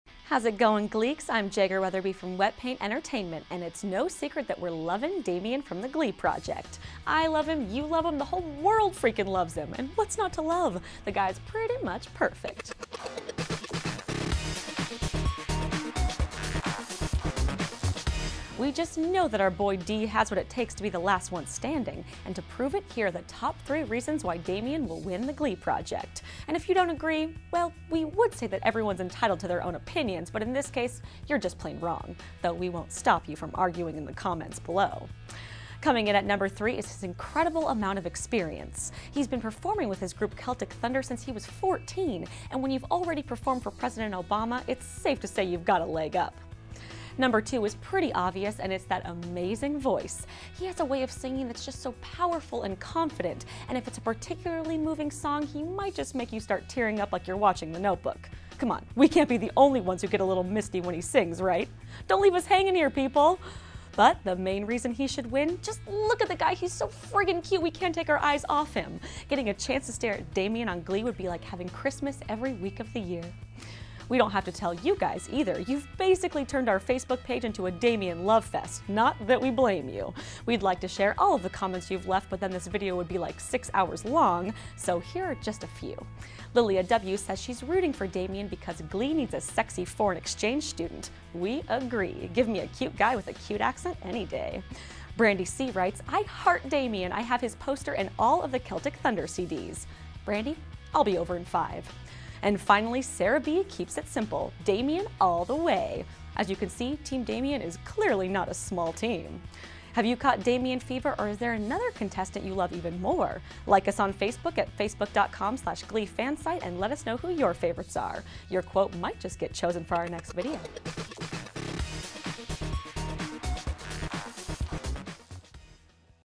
The Glee Project: 3 Reasons Why Damian McGinty Jr Will Win
- Visit us for more Glee exclusives, gossip, and spoilers!

How's it going, Gleeks? I'm Jager Weatherby from Wetpaint Entertainment, and it's no secret that we're loving Damian from The Glee Project. I love him, you love him, the whole world freakin' loves him. And what's not to love? The guy's pretty much perfect.

We just know that our boy D has what it takes to be the last one standing. And to prove it, here are the top 3 reasons why Damian will win The Glee Project. If you don't, agree... well, we would say that everyone's entitled to their own opinions, but in this case you're just plain wrong. Though we won't stop you from arguing in the comments below.

Coming in at number three is his incredible amount of experience. He's been performing with his group Celtic Thunder since he was 14. And when you've already performed for President Obama, it's safe to say you've got a leg up.

Number 2 is pretty obvious — and it's that amazing voice. He has a way of singing that's just so powerful and confident. And if it's a particularly moving song, he just might make you start tearing up like you're watching The Notebook. C'mon — we can't be the only ones who get a little misty when he sings, right? Don't leave us hanging here, people!

But the main reason he should win? Just look at the guy! He's so friggin' cute, we can't take our eyes off him. Getting a chance to stare at Damian on Glee would be like having Christmas every week of the year.

We don't have to tell you guys, either. You've basically turned our Facebook page into a Damian love fest — not that we blame you! We'd like to share all of the comments you've left, but then this video would be like six hours long. So here are just a few. Lilia W. says she's rooting for Damian because "Glee needs a sexy foreign exchange student." We agree — give me a cute guy with a cute accent any day! Brandy C. writes, "I heart Damian! I have his poster and all of the Celtic Thunder CDs." Brandy, I'll be over 5. And finally, Sara B. keeps it simple: "Damian all the wayy!!" And as you can see, Team Damian is clearly not a small team.

Have you caught Damian fever, or is there another contestant you love even more? and let us know who your favorites are. Your quote might just get chosen for our next video!